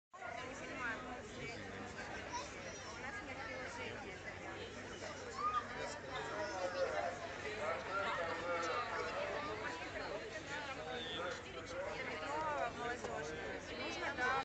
Толпа у метро "Молодёжная" после фестиваля "Круг Света"
Давка, все едут по домам. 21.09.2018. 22:45